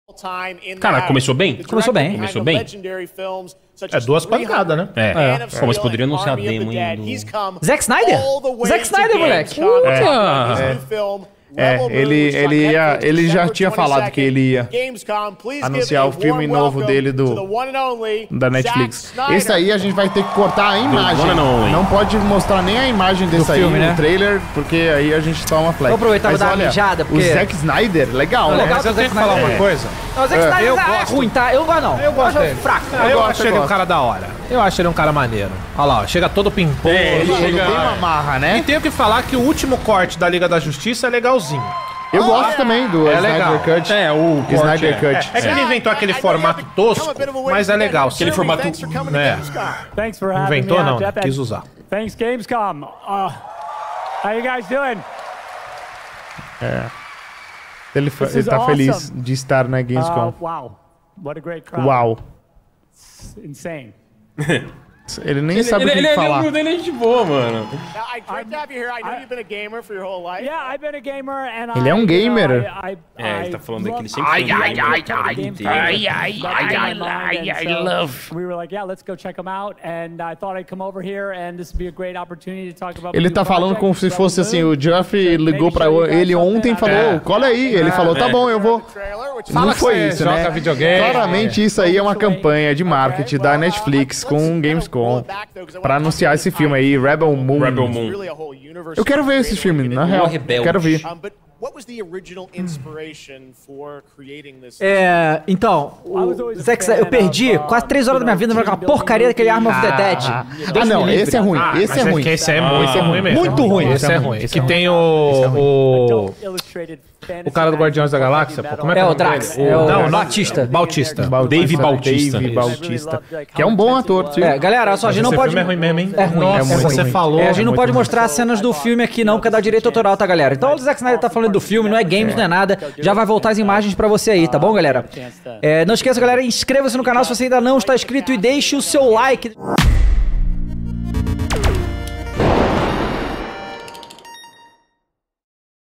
ZACK SNYDER apareceu num evento de GAMES
Fala galera! No vídeo de hoje teremos um corte comentando sobre O NOVO filme do Zack Snyder na Netflix.